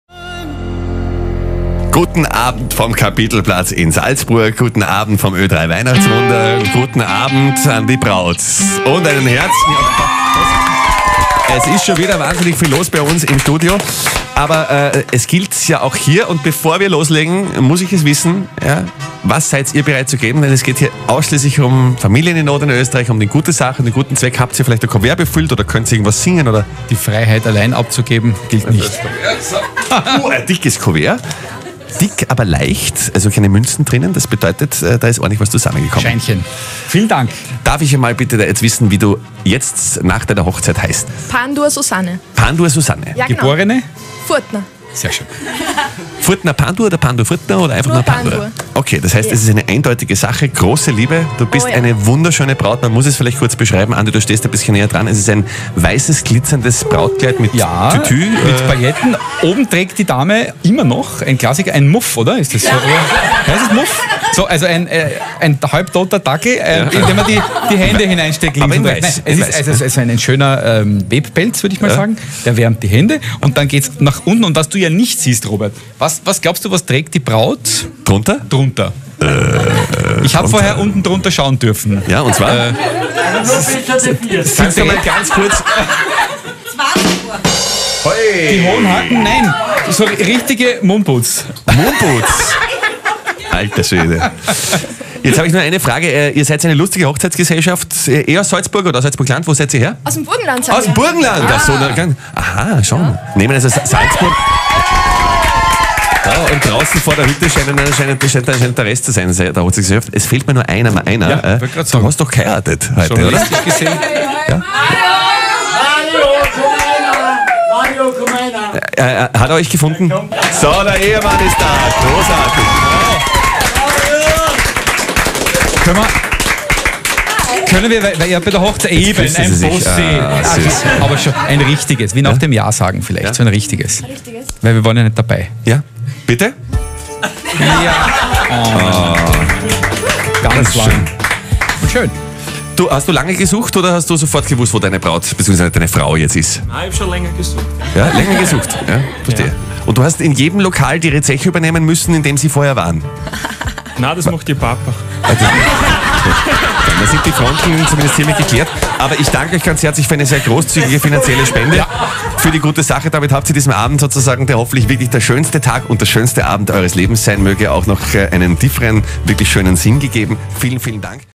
Andi und Robert und die gestohlene Braut
Wohin in Salzburg mit einer gestohlenen Braut? Das gläserne Ö3-Studio am Salzburger Kapitelplatz ist vielleicht nicht der beste Platz, um sie zu verstecken - aber möglicherweise der originellste.